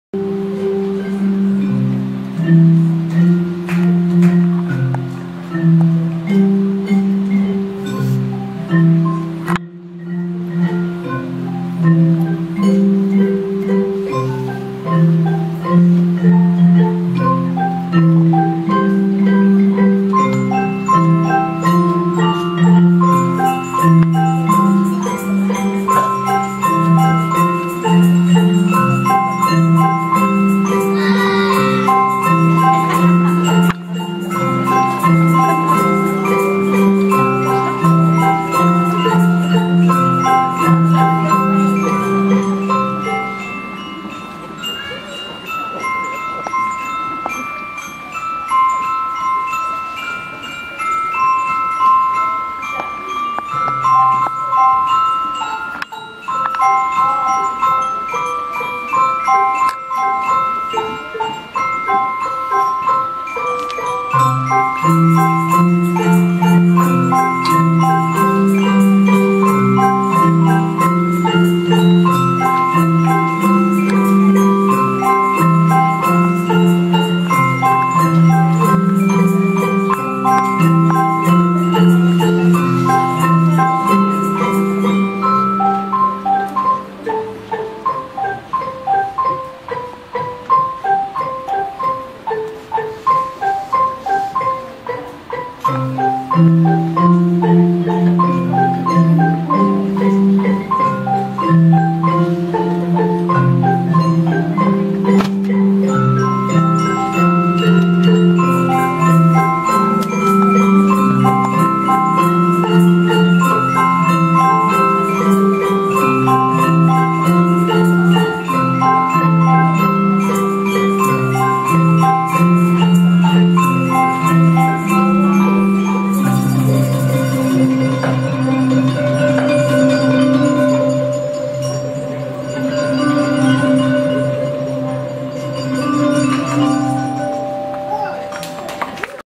ORF2
Brookshire Elementary's ORF Ensemble's concert.
Spring, 2014